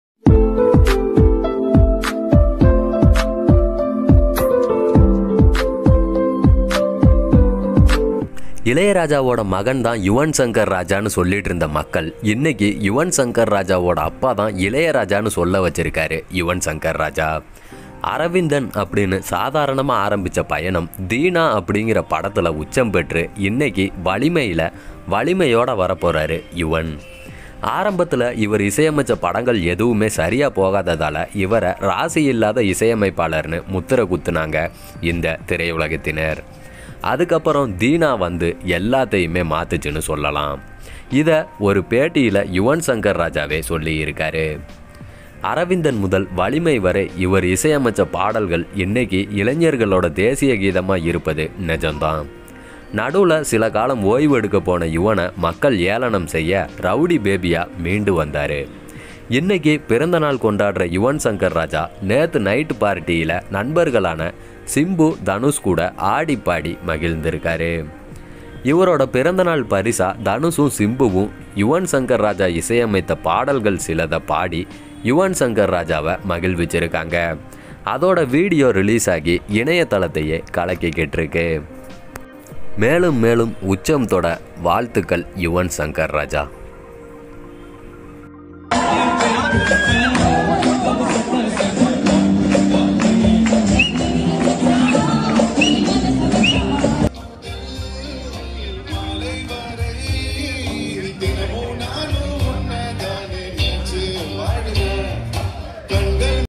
Simbu And Dhanush Singing In Yuvan Shankar Raja Birthday Party | U1 | Red Spider
CHANNEL DESCRIPTION